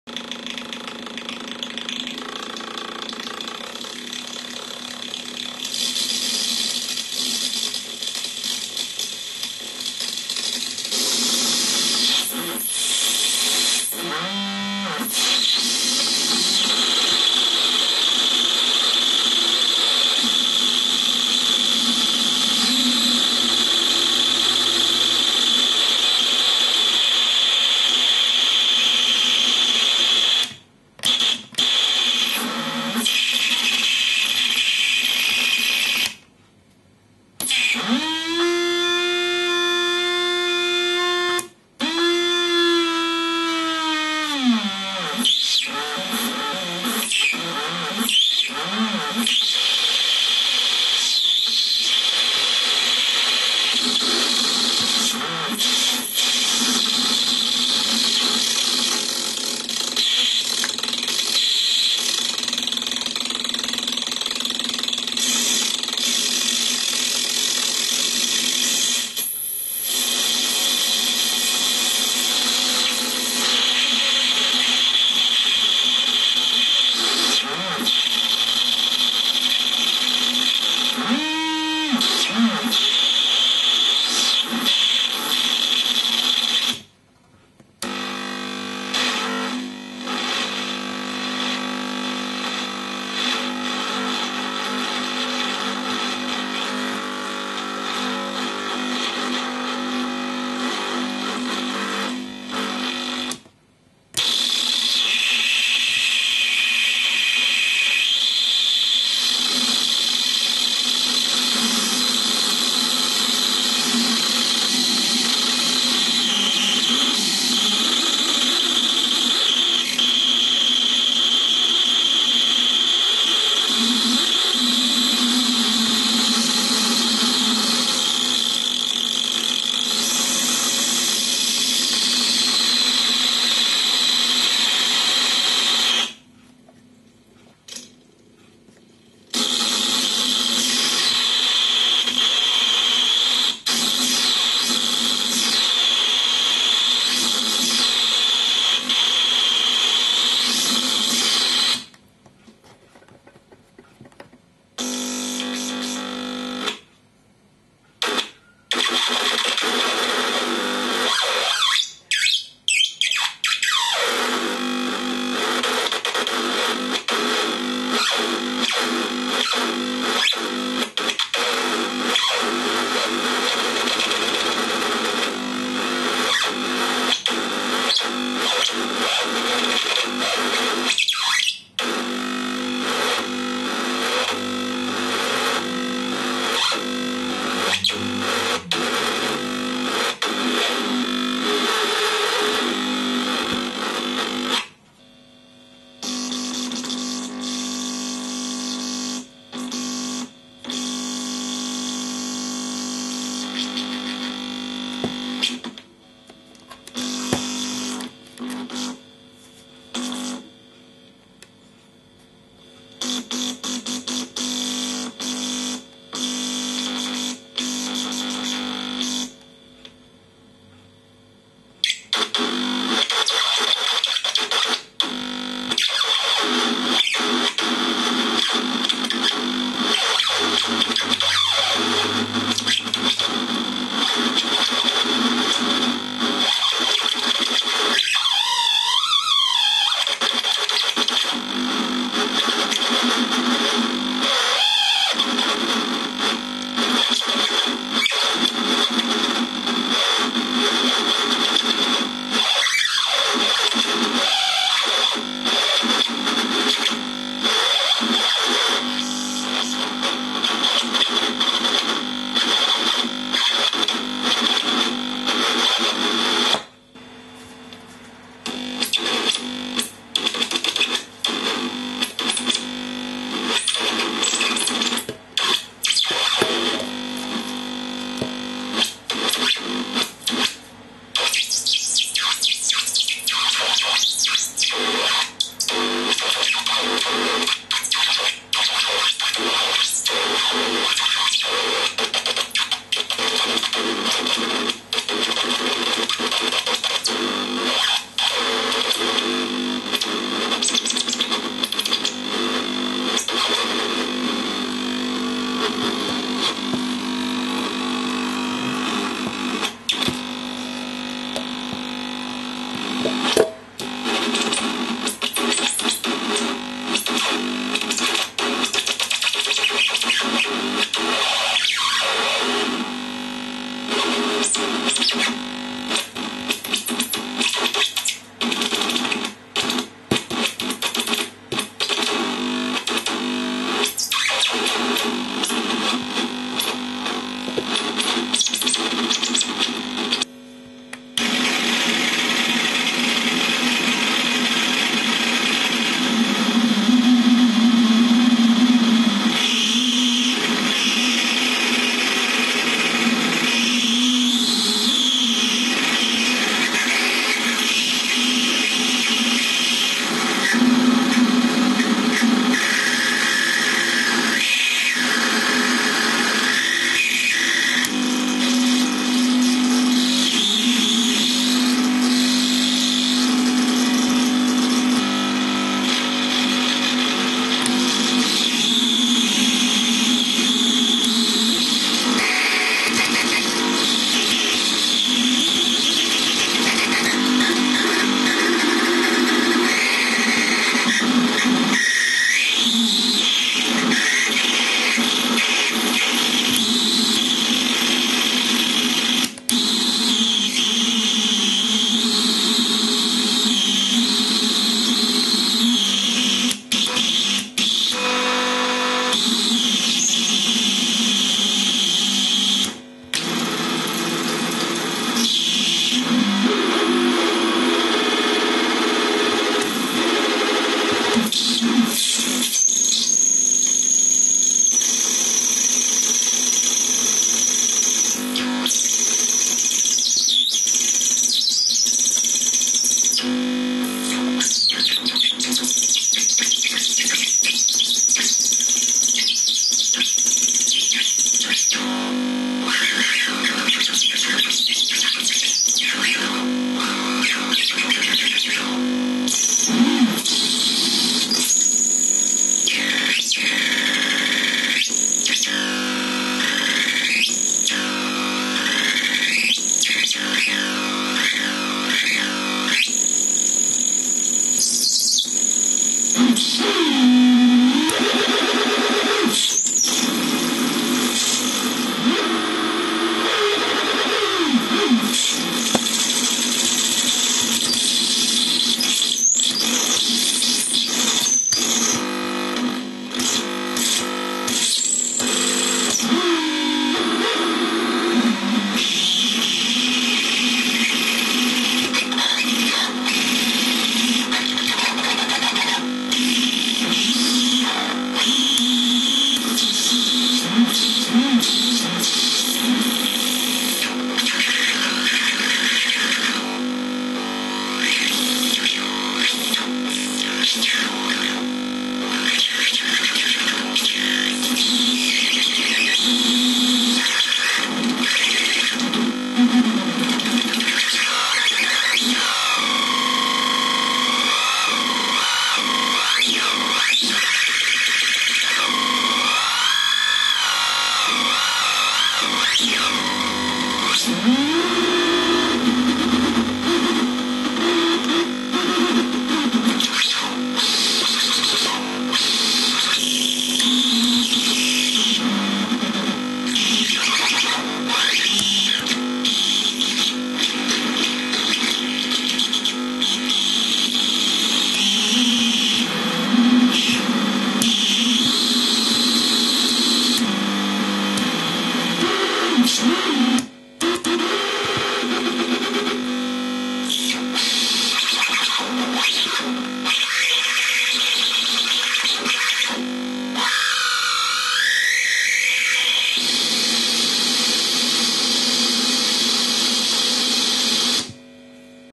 PYE Radio Bend
Kapootchnik I,. Model: PYE - ECR36  finished 12 Dec '09. Good for droney wefty noise. Found a strong circuit that can run through body(s) and metallic things woot. Up to three bodies, two bodies= variable sensitivity to the touch. Through water not so much, through Gatorade? will try soon. Cheap low amp switches (cheaper than toggles) but more effort in filing..